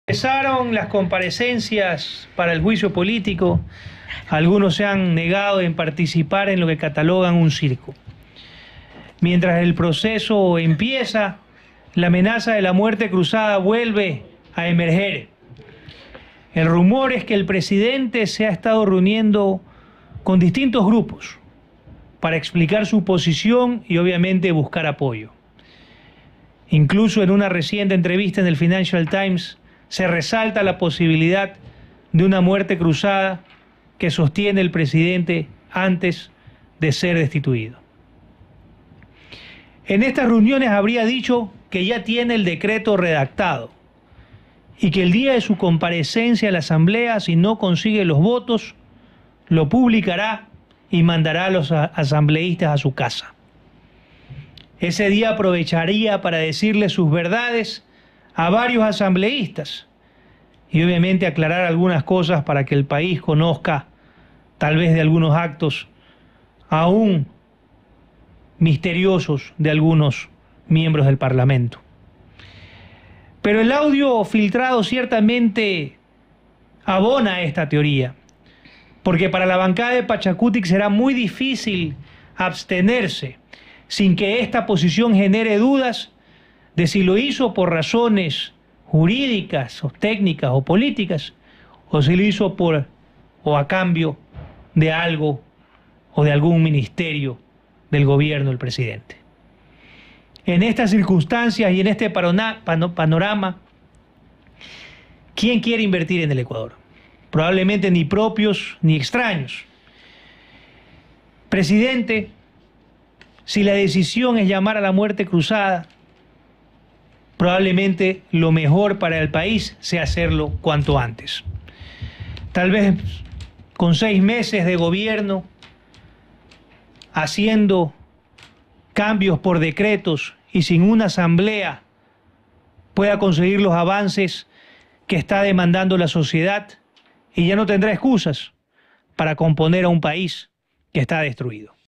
Editorial 19 de Abril de 2023
🗣️ "Mientras el proceso de JuicioPolíticoALasso empieza, la amenaza de la MuerteCruzada vuelve a emerger... El audio filtrado (de Cecilia Velasque) abona a esta teoría".
O
Mira el editorial de LaDefensaEc por Gabriel Martínez.